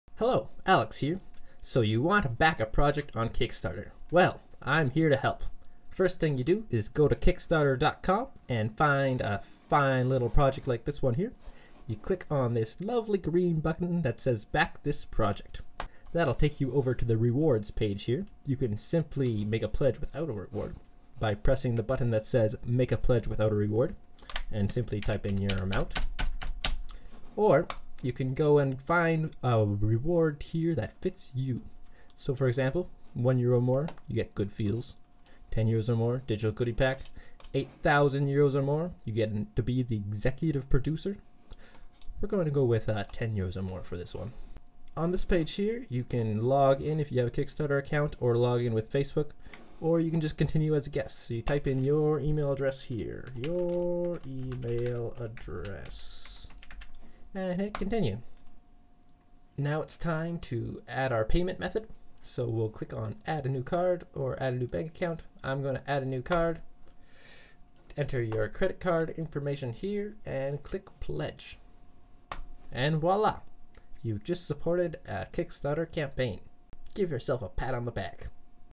How to Back a Project on Kickstarter
If you're new to Kickstarter, this should help you get started.

The steps:

1) Find a project you like and click the big, green "Back this project" button.

2) Choose your reward tier, or make a pledge without a reward by clicking the radio button to the left of your selection. You can always increase the amount of your pledge.

4) Add your payment information. Choose between "Add a new card" and "Add a new bank account" When you've made your selection, just fill in your information and press the "Pledge" button.

That's all there is to it. If you'd like, you can practice by supporting our kickstarter campaign